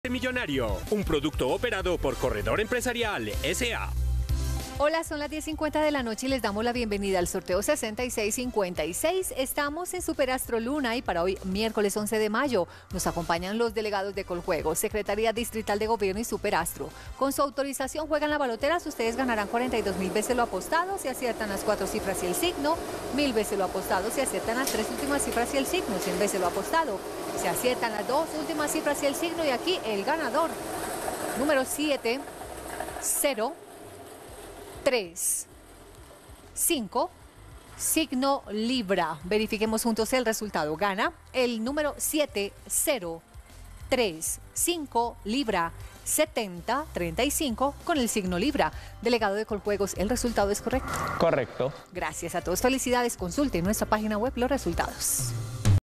ASTRO LUNA Resultado del MIÉRCOLES 11 DE MAYO de 2022 | SUPER ASTRO ✅🥇🔥💰
Hola buenas noches, te presentamos los resultados de las loterías y chances en Colombia, vamos con el Resultado para SUPER ASTRO LUNA del MIÉRCOLES 11 DE MAYO de 2022.